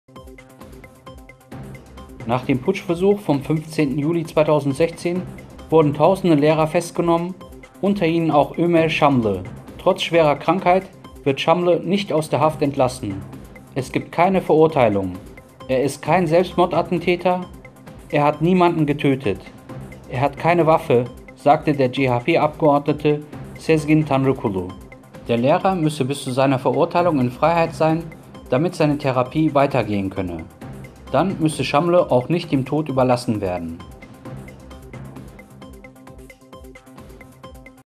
SCHWERKRANKER LEHRER WIRD DURCH HAFT DEM TOD ÜBERLASSEN
ach dem Putschversuch vom 15. Juli 2016 wurden Tausende Lehrer festgenommen, unter ihnen auch Ömer Şamlı. Trotz schwerer Krankheit wird Şamlı nicht aus der Haft entlassen. „Es gibt keine Verurteilung. Er ist kein Selbstmordattentäter. Er hat niemanden getötet. Er hat keine Waffe;“ sagte der CHP-Abgeordnete Sezgin Tanrıkulu. Der Lehrer müsse bis zu seiner Verurteilung in Freiheit sein, damit seine Therapie weitergehen könne. Dann müsse Şamlı auch nicht dem Tod überlassen werden.